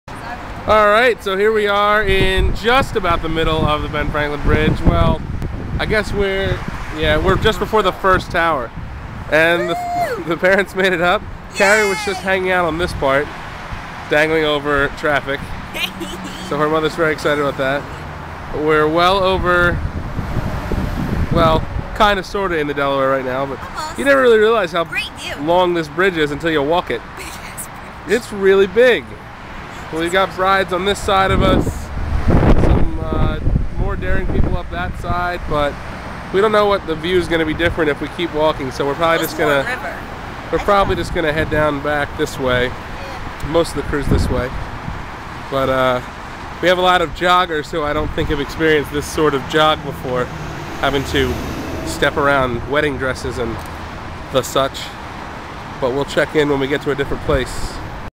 Philly TTD: Mom & Dad & us on the bridge!!
Mother's Day Philly Trash Bash, from a groom's perspective.